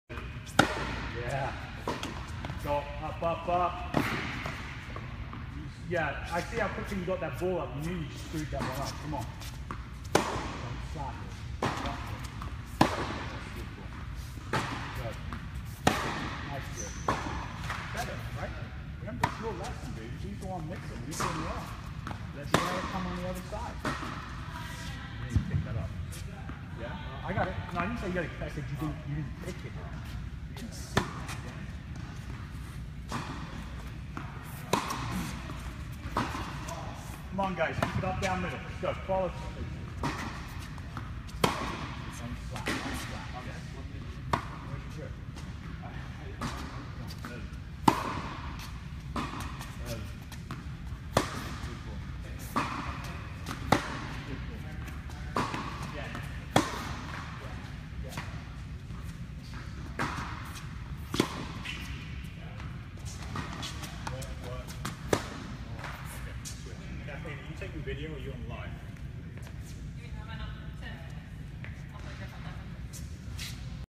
Coach Tim with Marshall and Salvador – Part 1
Blue Chip Tennis Coach Tim Bainton doing a lesson with Marshall and Salvador. Blue Chip Tennis Academy offers lessons, clinics, camps, tournaments and more! Learn and play USTA Mid-Atlantic tennis from the best